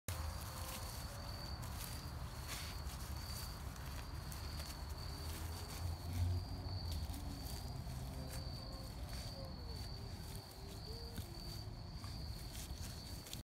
Video of adoptable pet named Flynn 2266
Flynn 2266 is an adoptable pet with Shenandoah Shepherd Rescue Inc in Stephens City VA.